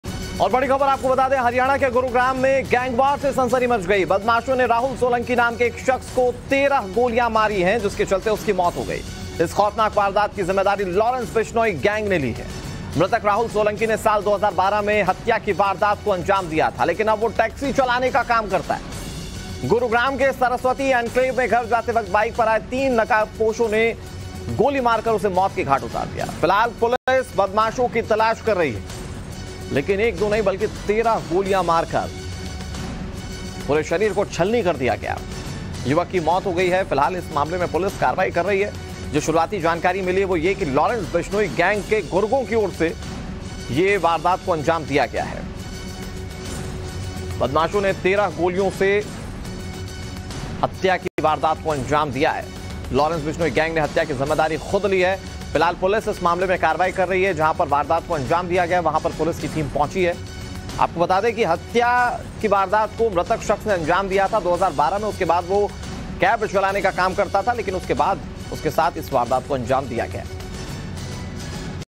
Breaking News: Haryana के Gurugram में गैंगवार से सनसनी, बदमाशों ने युवक की गोली मारकर की हत्या
Breaking news | हरियाणा के गुरुग्राम में गैंगवार से सनसनी मच गई है। बदमाशों ने राहुल सोलंकी नाम की एक शख्स की हत्या कर दी है। बता दें कि इस हत्या की जिम्मेदारी लोरैंस बिश्नोई गैंग ने ली है। देखिए Times Now Navbharat पर पूरी खबरें..